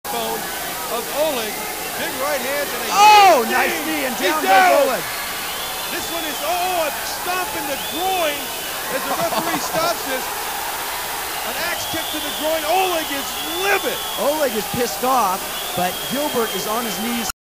Schrijber with axe kick to groin
Old school bare knuckle fight in St. Petersburg, Russia